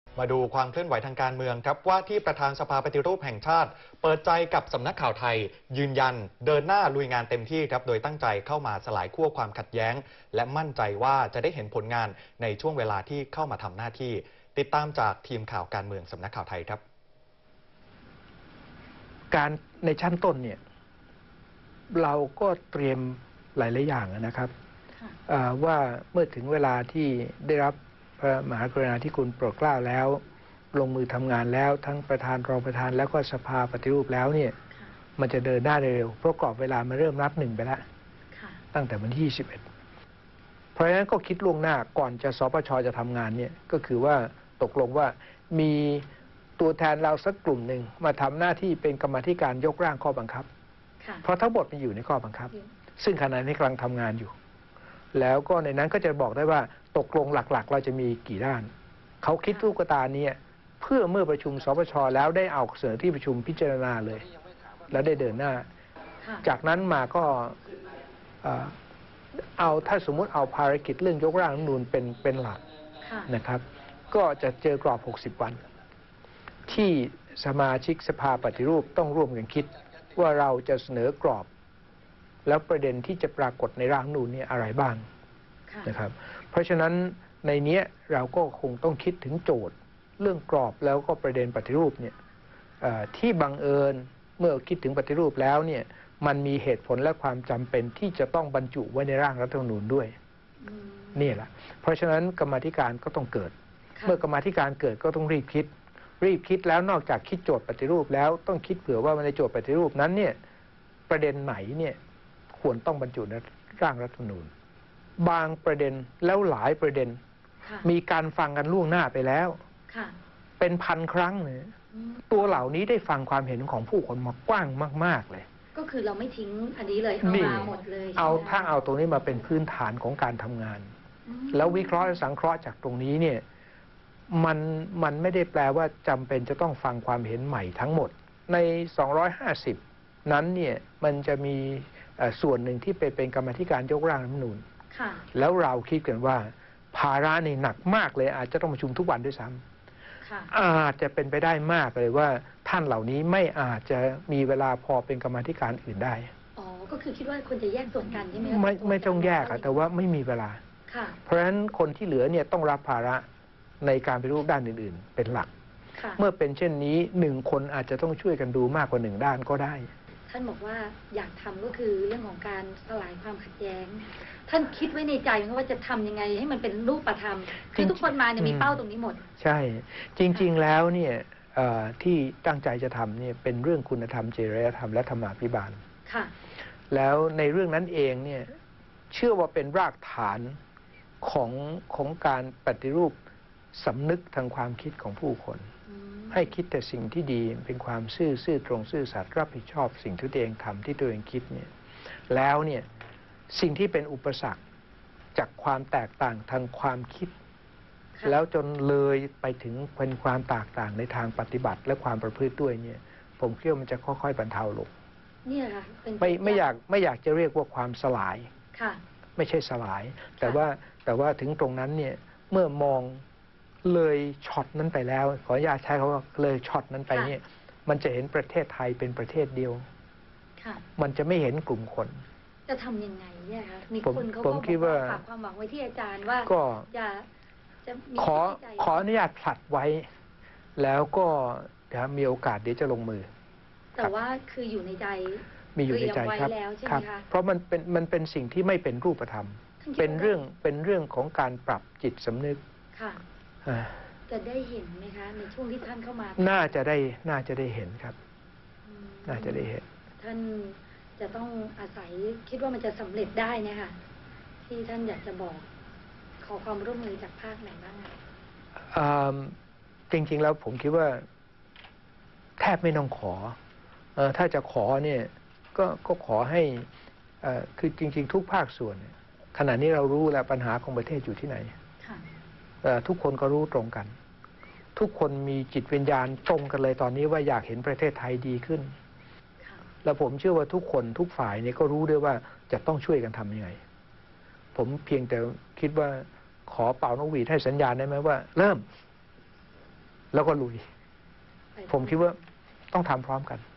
MCOT : สัมภาษณ์พิเศษ “เทียนฉาย” เปิดใจลุยงาน สปช. 25/10/2557
รัฐสภา 25 ต.ค. – “เทียนฉาย” เปิดใจ เดินหน้าลุยงาน สปช. สานต่อข้อมูลจากหน่วยงานต่างๆ ที่เคยทำไว้  ไม่จำเป็นต้องเริ่มใหม่ มั่นใจจะทำไทยเป็นหนึ่งเดียว สลายความขัดแย้ง เชื่อ ทุกฝ่ายพร้อมร่วมมือ เพียงแค่รอสัญญาณเริ่มต้น